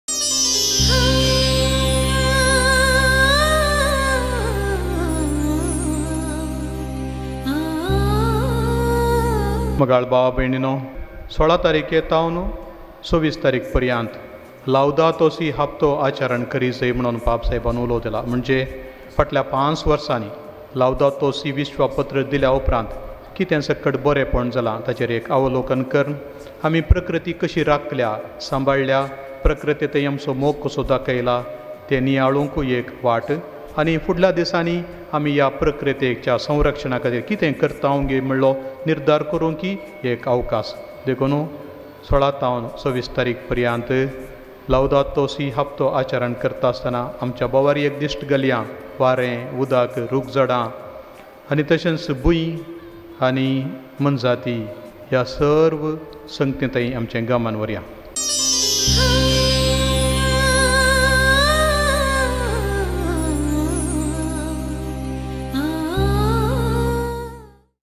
Laudato Si' Week ( 16 - 24 May 2020) : Message from Bishop Of Mangalore
Produced by Canara Communication Centre Diocese of Mangalore.